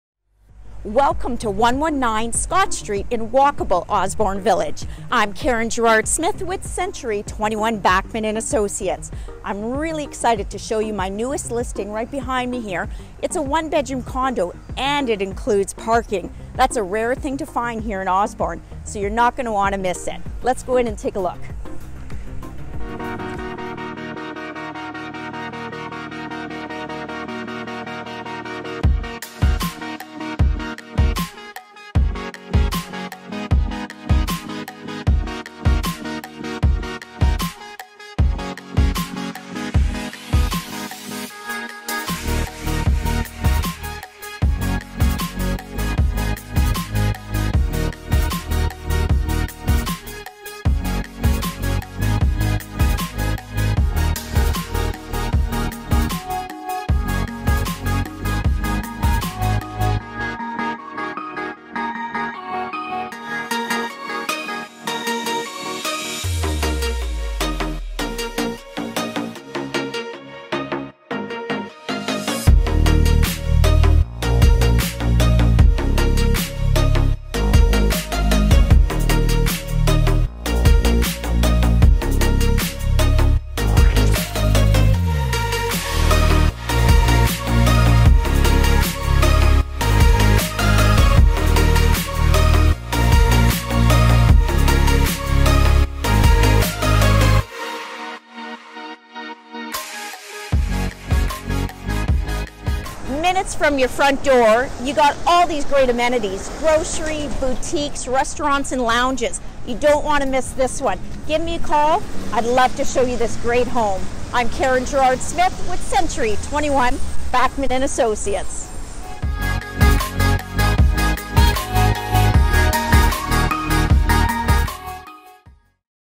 SOLD: 10-119 Scott Street | Karen Girard-Smith Winnipeg Realtor
Chic, condo living, in the heart of Osborne Village...what more could you ask for!
Featuring a personal parking spot, walking distance to amenities, rapid transit right around the corner and so much more!

This cozy, one bedroom condo has an updated design and a fresh new look which includes new flooring, paint, and more. Your new home features a full spacious kitchen, four-piece bathroom, large bedroom, ample amount of storage solutions and A HIGHLY DESIRABLE PARKING SPACE. Located in one of Winnipeg’s most desirable neighbourhoods, Osborne Village, you are minutes away from rapid transit and numerous bus routes. Osborne Village offers a trendy atmosphere of new and local businesses, including restaurants, bars, boutique shops, grocery and so much more. The Village presents a vibrant arts scene and a tight knit sense of community. Hurry you don't want to miss this one!!!

⭐️ARE YOU READY TO BUY OR SELL YOUR NEXT HOME?⭐️